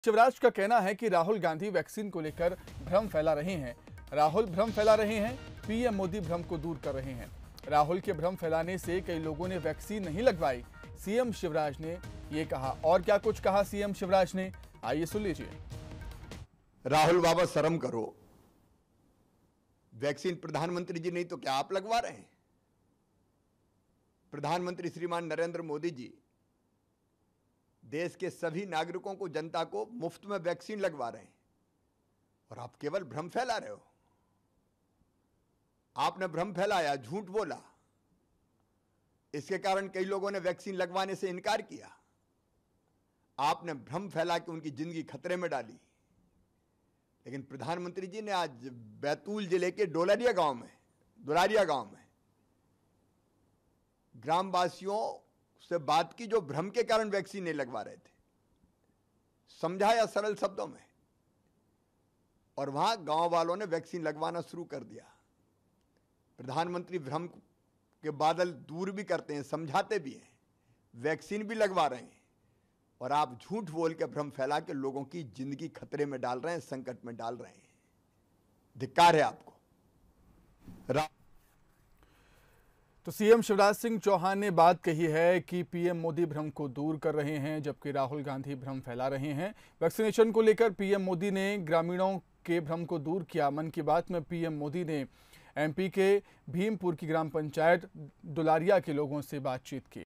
बड़ी खबर। राहुल गांधी वैक्सीन को लेकर भ्रम फैला रहे है- CM शिवराज
राहुल भ्रम फैला रहे है, PM मोदी भ्रम दूर कर रहे है: शिवराज
राहुल के भ्रम फैलाने से कई लोगों ने वैक्सीन नहीं लगवाई:शिवराज
राहुल ने कई लोगों की जिंदगी खतरे में डाली: शिवराज
पीएम ने आज बैतूल जिले के डुलारिया गांव में ग्रामीणों से बात की: शिवराज
पीएम ने डुलारिया के ग्रामीणों का भ्रम दूर किया: शिवराज
VIDEO CREDIT- DD NEWS